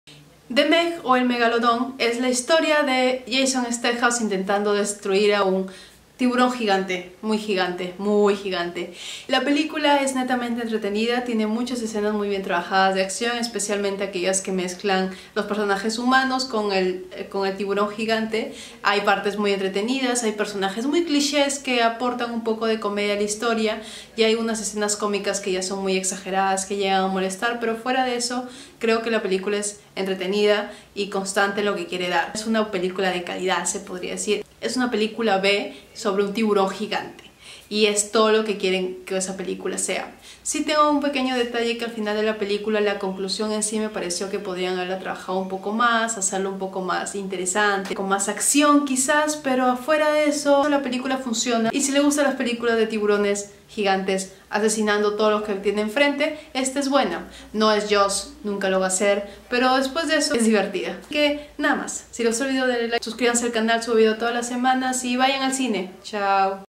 Reseña de Megalodón
Es un hombre contra un tiburón prehistórico gigantes

Y ustedes ¿ya vieron la película? ¿les gustó? ¿no les gustó?

Espero disfruten el vídeo.
Se aceptan sugerencias y opiniones.